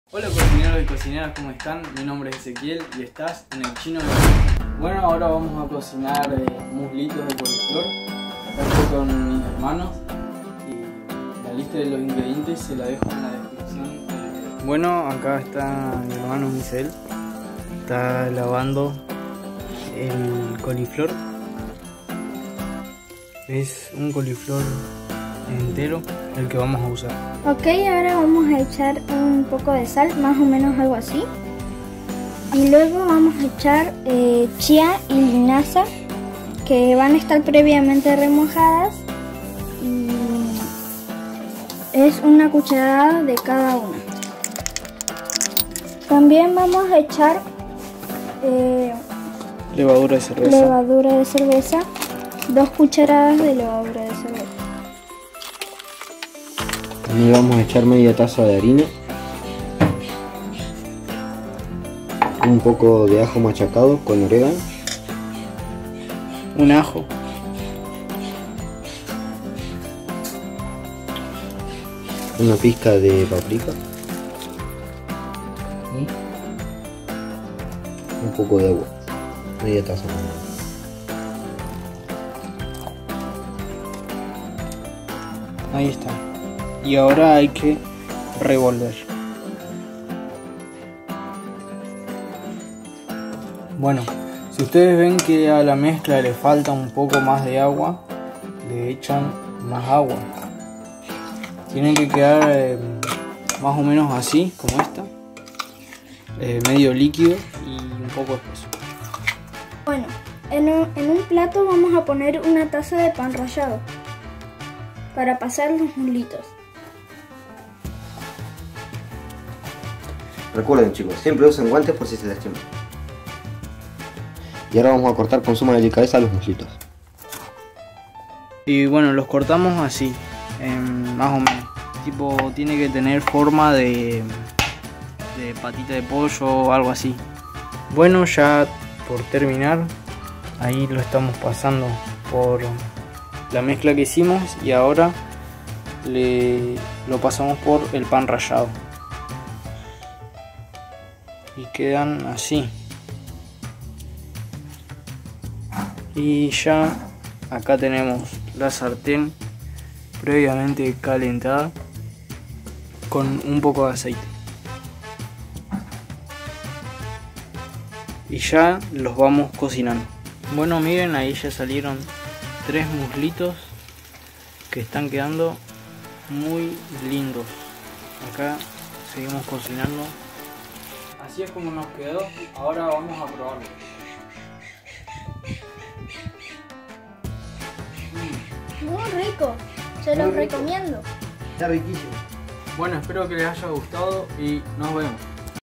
( ͡❛ ͜ʖ ͡❛)Cocinando con mis Hermanos unos Ricos y Faciles Muslitos de Coliflor!!!
En este video estoy cocinando unos deliciosos y fáciles muslos de coliflor con mis hermanos.

Las coles son una verdura deliciosa y fácil de cocinar, y a mis hermanos y a mí nos encantan. ¡Míranos cocinar estos muslos de coliflor en este video y comprueba por ti mismo lo fáciles y deliciosos que son!

INGREDIENTES
              I
              I
              I
       \     I     /
1 Coliflor o brocoli
sal
2 Cucharadas de levadura nutricional (OPCIONAL)
1 Diente de ajo machacado con una pisca de oregano
Media taza de agua
1 Taza de pan rallado
1 Pizca de curcuma o paprika
1 Cucharada de chia y lino,previamente hidratados en 10 cucharadas de agua
Media taza de harina de trigo o de avena triturada